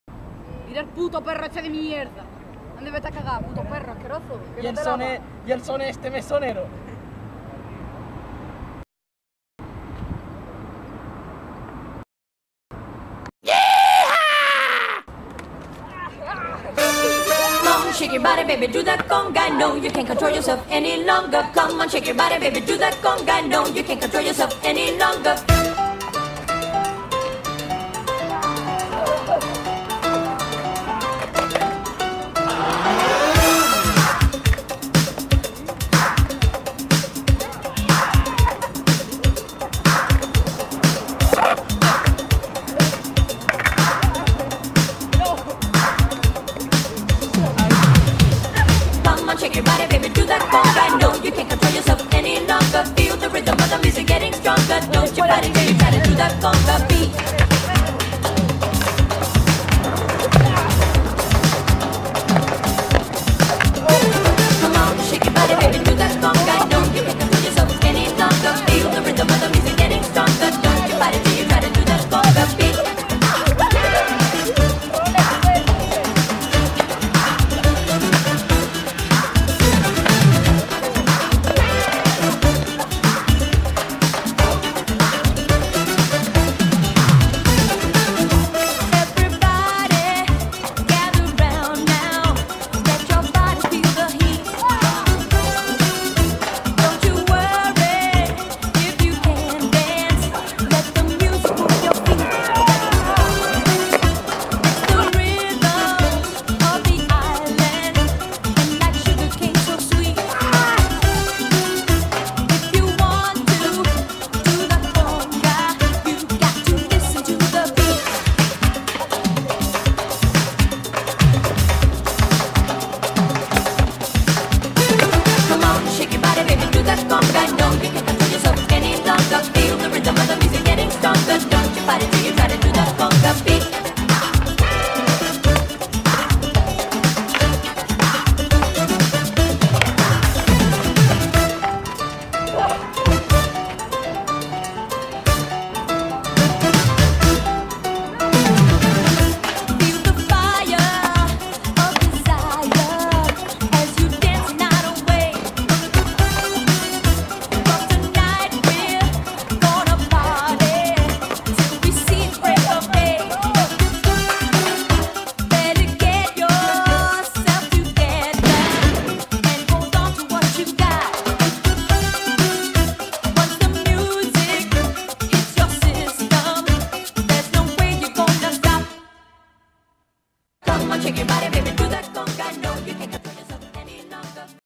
Pelea ilegal Dani vS sone
Pelea ilegal en el catavino de sone y dani. Jajajaja